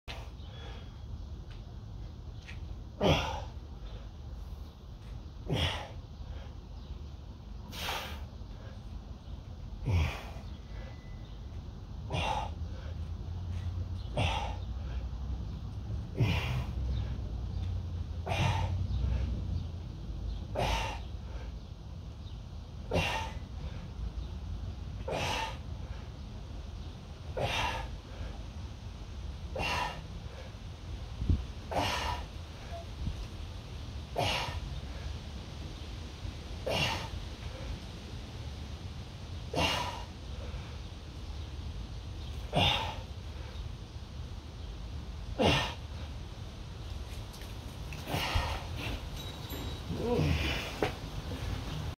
20
Trying various hand/arm placements. Tucking my elbows too much hurts my shoulder.